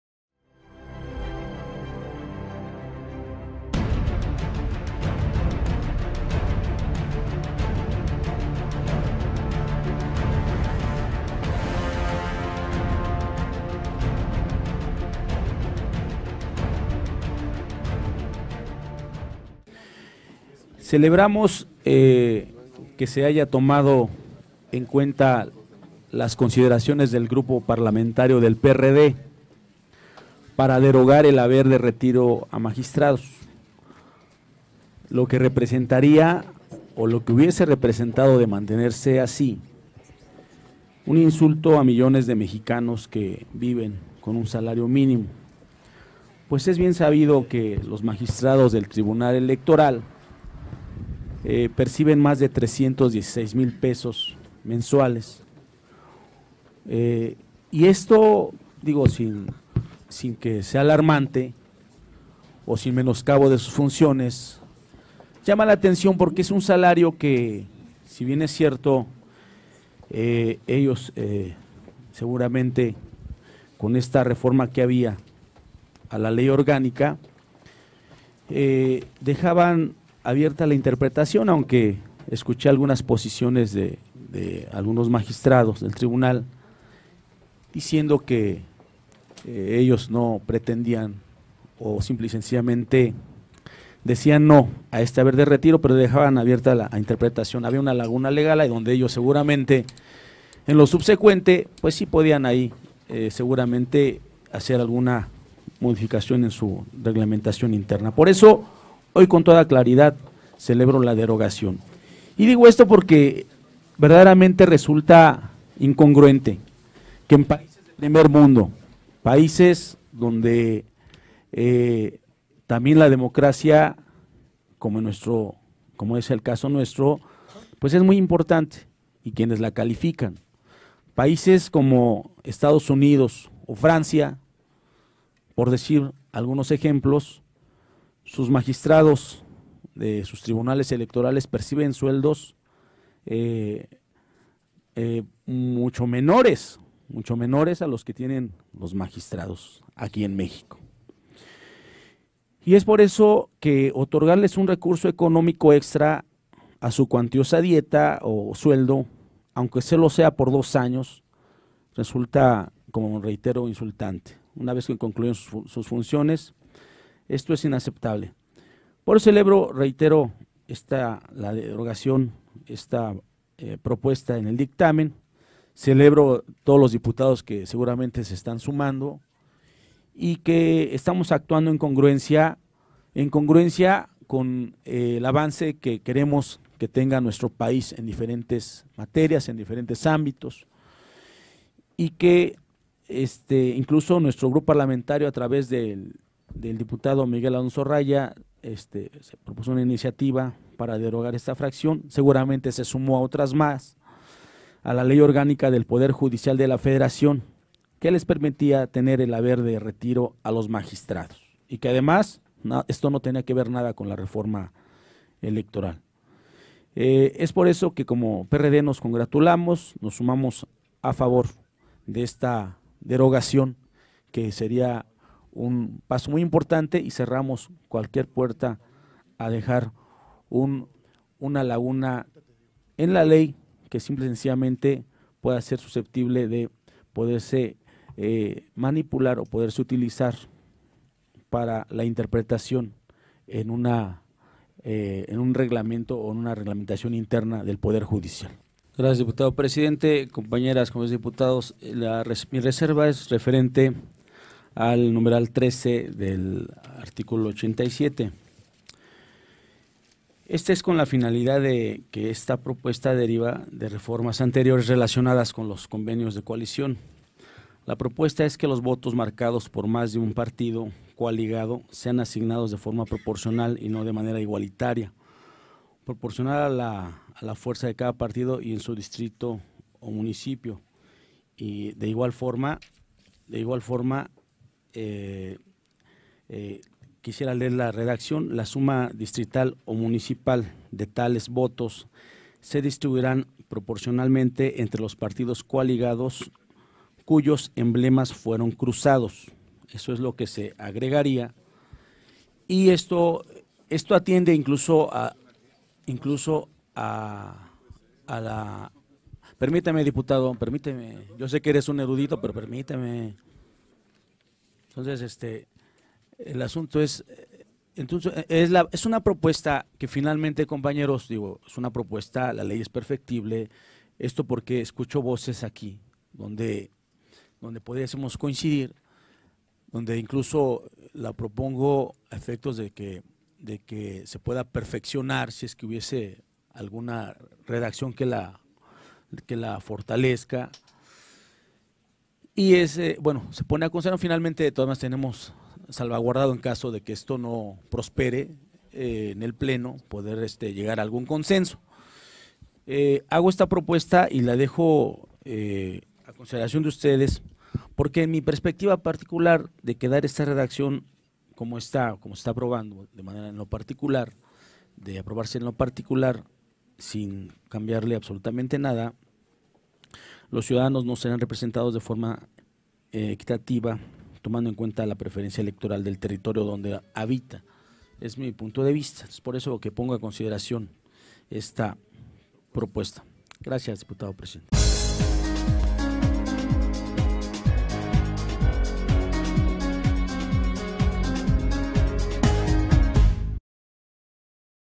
Intervención del diputado Julio César Moreno
CELEBRA JULIO CÉSAR MORENO QUE DIPUTADOS DEL PRI DIERAN MARCHA ATRÁS AL HABER DE RETIRO A MAGISTRADOS DEL TRIFE

·         Esta decisión obedece a la insistente presión del PRD y a la indignación de la clase trabajadora, que vive con un salario mínimo

·         Era insultante el haber de retiro a funcionarios que perciben más de 316 mil pesos mensuales, cifra superior a magistrados de países primermundistas como Estados Unidos o Francia

El diputado federal y presidente de la comisión de Puntos Constitucionales, Julio César Moreno Rivera, celebró que legisladores del Partido Revolucionario Institucional (PRI) hayan dado marcha atrás al haber de retiro a magistrados del Tribunal Electoral del Poder Judicial de la Federación (TEPJF).

Esta decisión obedece a la insistente presión del PRD y a la indignación de la clase trabajadora, que vive con un salario mínimo, por lo que no era factible un haber de retiro millonario a servidores públicos que perciben más de 316 mil pesos mensuales, cifra superior a magistrados de países primermundistas como Estados Unidos o Francia.

La iniciativa propuesta por el partido gobernante y cabildeada por propios magistrados, en el Senado de la República, ponía en entredicho la imagen de magistrados del TEPJF que mostraron su beneplácito al enterarse que se había aprobado una millonaria e insultante pensión vitalicia, a pesar de que la ciudadanía cuestionó mucho su labor en las pasada elecciones, aseguró Julio César Moreno.

Agregó que lo anterior se prestó a interpretaciones de varios analistas, académicos y la opinión pública, que afirmaban que se trataba de un pago anticipado de favores, hecho que tenía que retirarse por ser un agravio social y un albazo legislativo.

CPLGPPRD

---o0o---